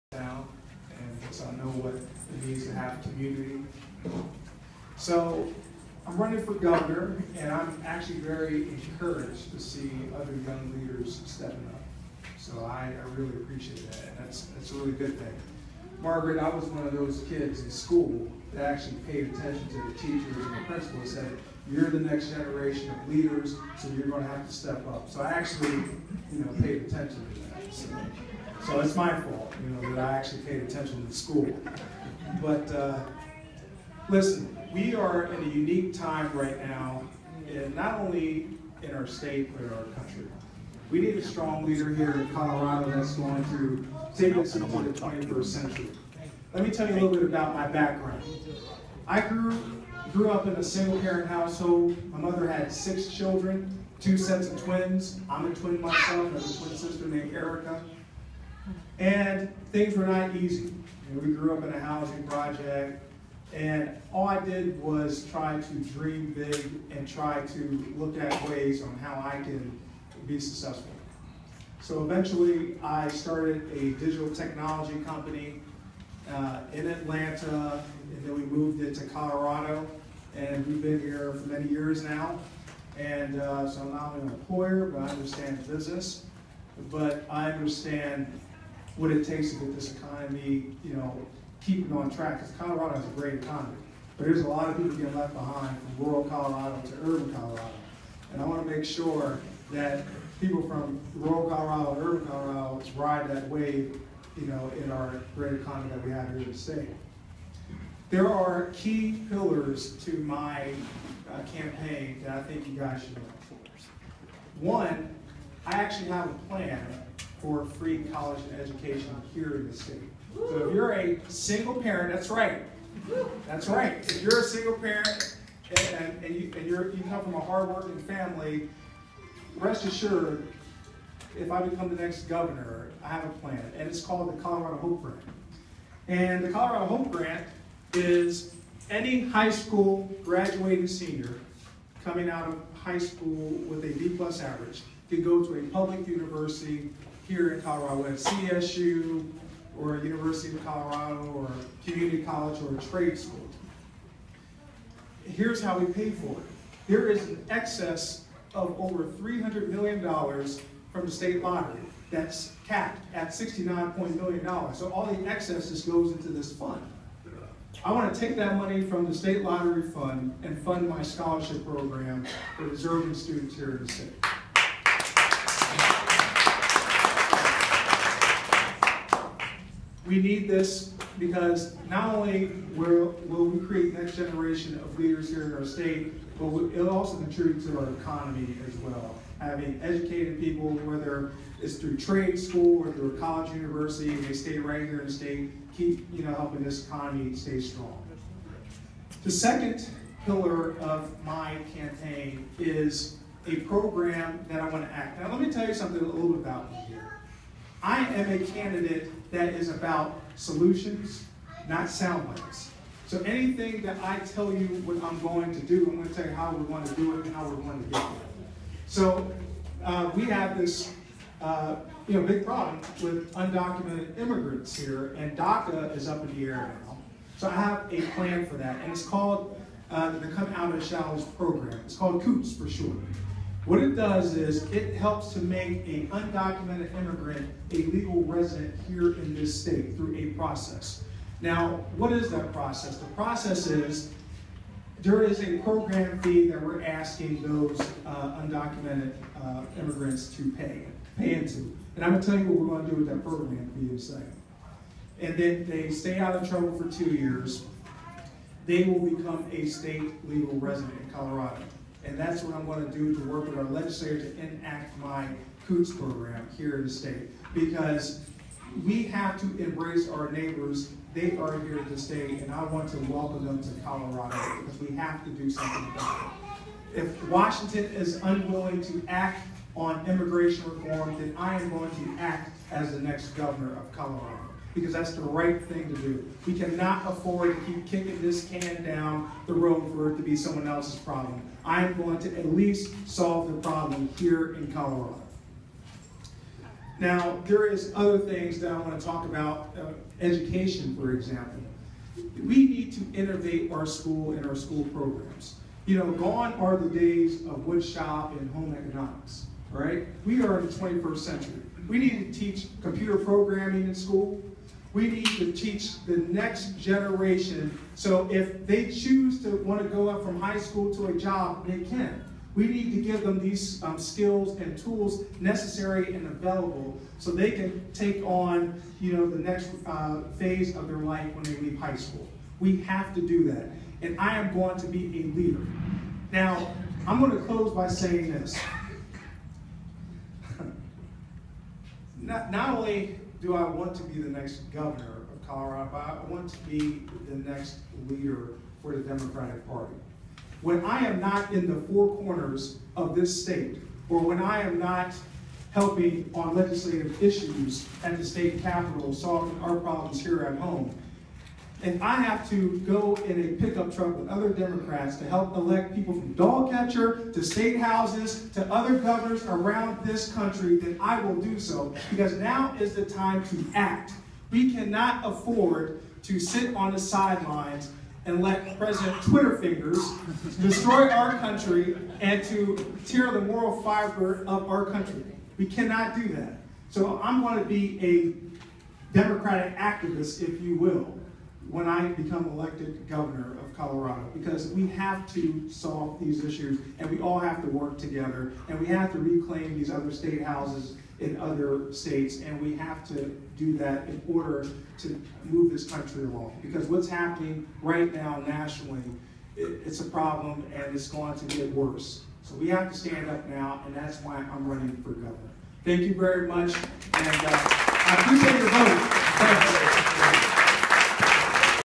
Erik Underwood in Pueblo, Colorado
Erik Underwood attended a Democratic County picnic in Pueblo, Colorado and gave a speech on his vision for Colorado. September 9th, 2017. Underwood for Colorado